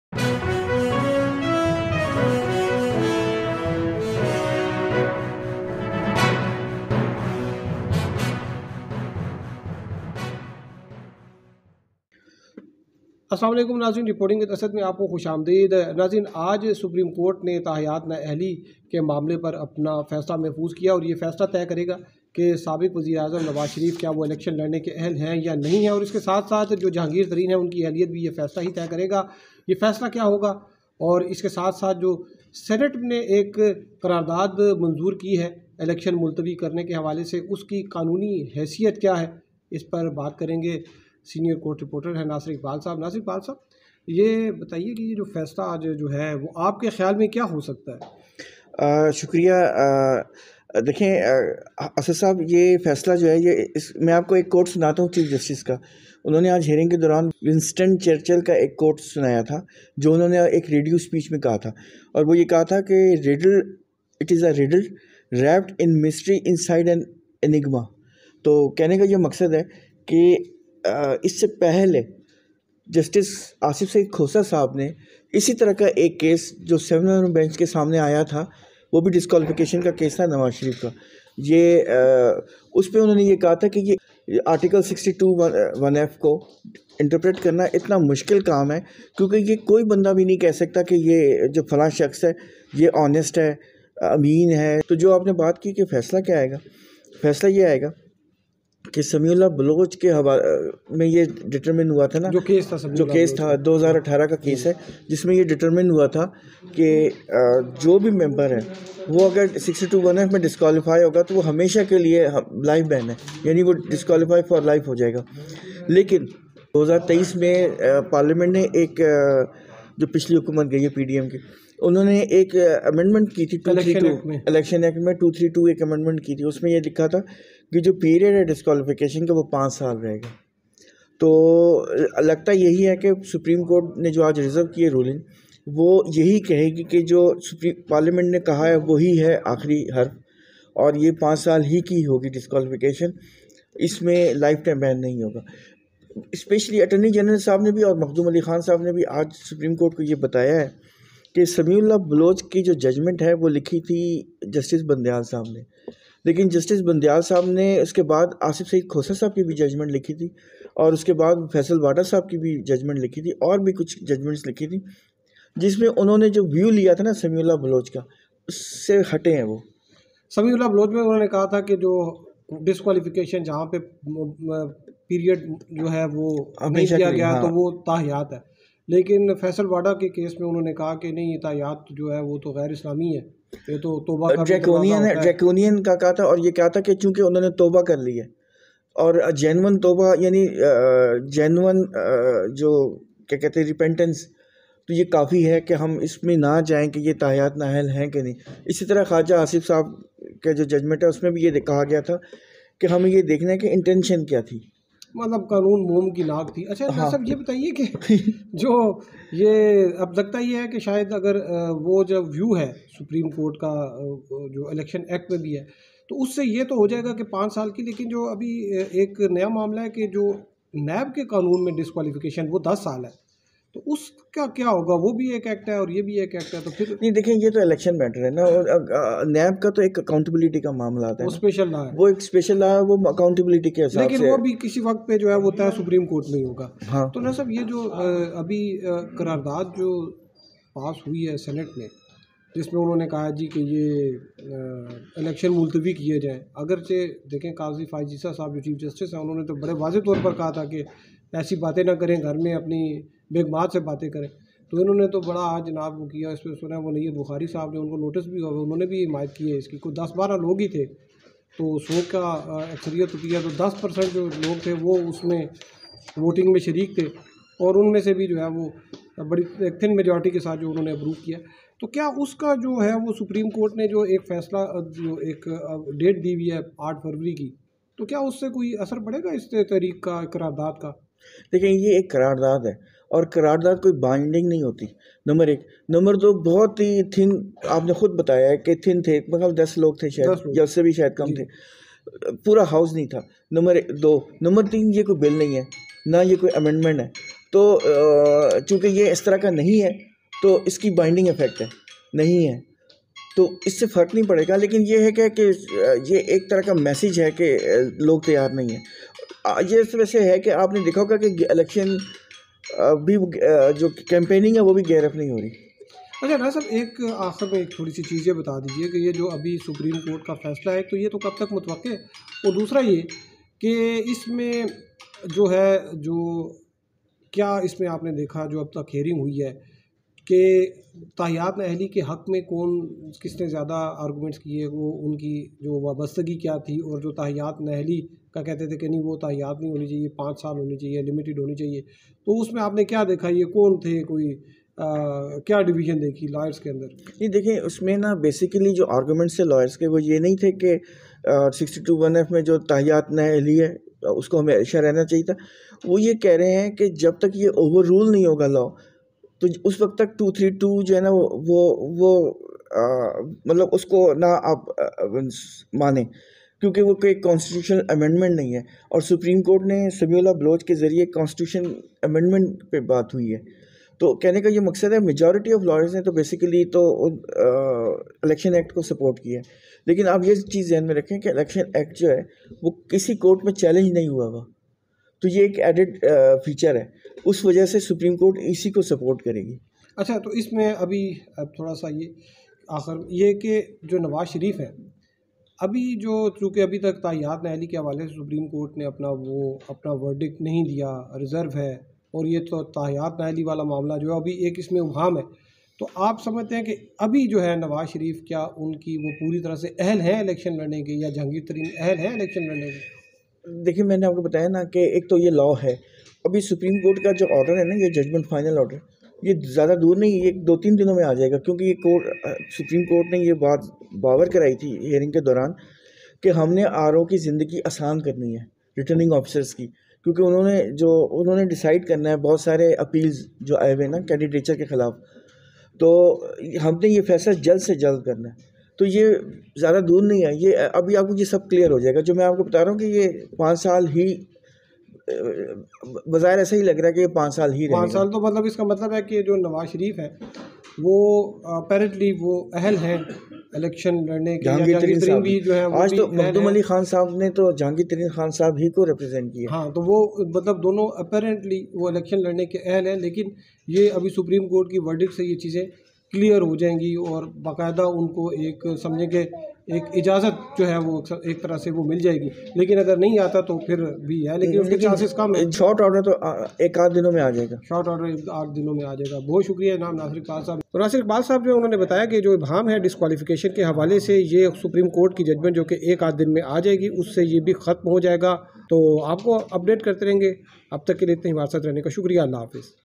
SUPREME COURT DECISION ON LIFETIME DISQUALIFICATION, SENIOR JOURNALIST NASIR IQBAL SPILLS THE BEANS
تاحیات نااہلی پر سپریم کورٹ کا فیصلہ، سینئر کورٹ رپورٹر ناصر اقبال نے اندر کی بات بتا دی، الیکشن ملتوی کرنے کی سینیٹ قرارداد کی قانونی حیثیت کیا ہے؟ تفصیلات جانئے